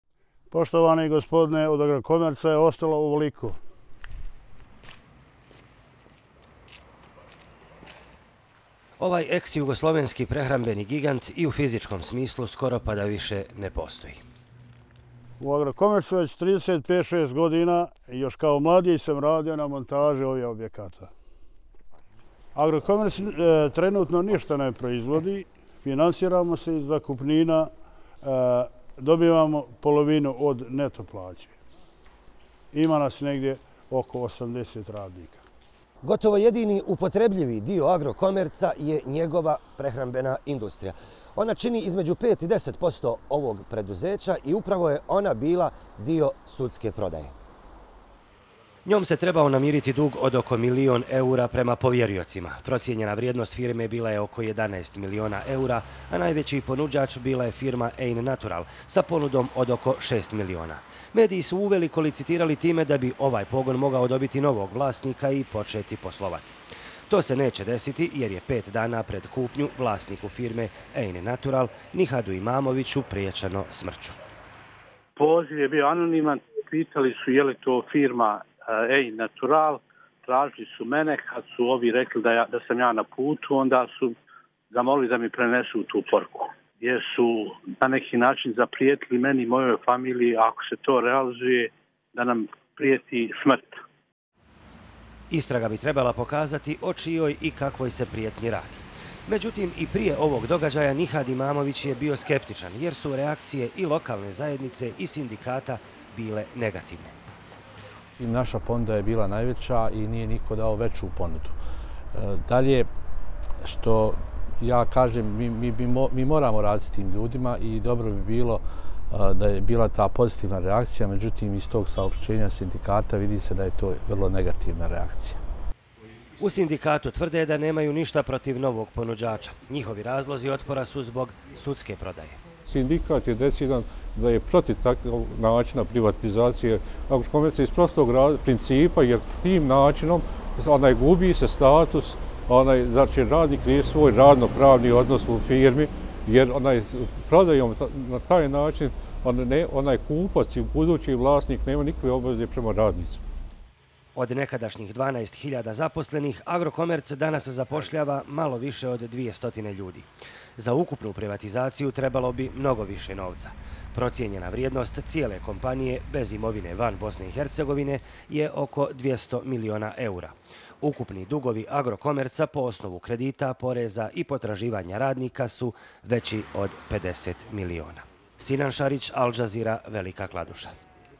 Imamović zbog prijetnji smrću odustaje od Agrokomerca?
Jedan od sektora nekadašnjeg privrednog giganta iz Velike Kladuše - "Agrokomerca", krajem prošlog mjeseca na trećoj javnoj ponudi postigao je maksimalnu cijenu od gotovo 6 miliona eura. Ipak, najozbiljniji potencijalni kupac nekoliko dana pred istek licitacionog roka, želi da odustane od ponude zbog prijetnji smrću koje je dobio.